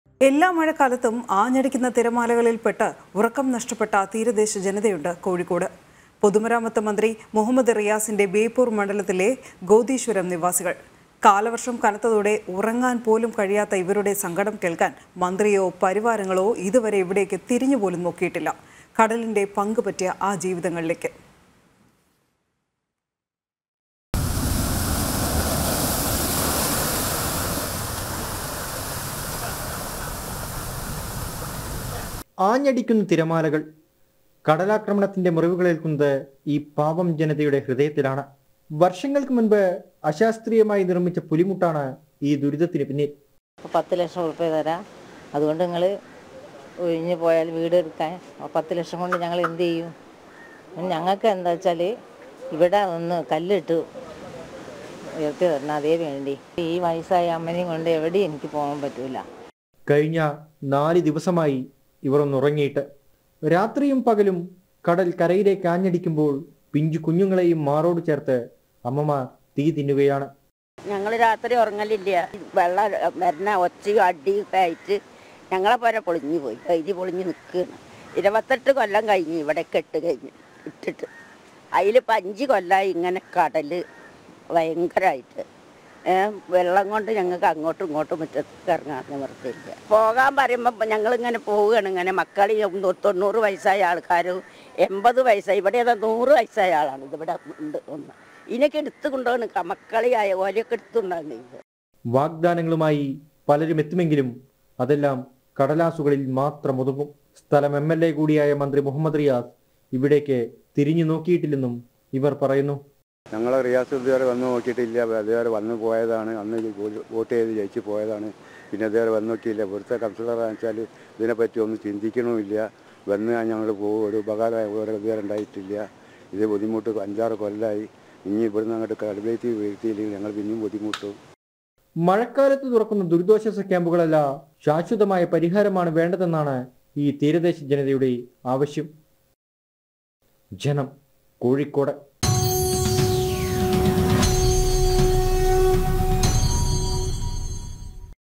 എല്ലാ മഴക്കാലത്തും ആഞ്ഞടിക്കുന്ന തിരാമലകളിൽപ്പെട്ട് ഉറക്കം നഷ്ടപ്പെട്ട തീരദേശ ജനതയുണ്ട് കോഴിക്കോട്
NEWS ANCHOR : ANU AURAJ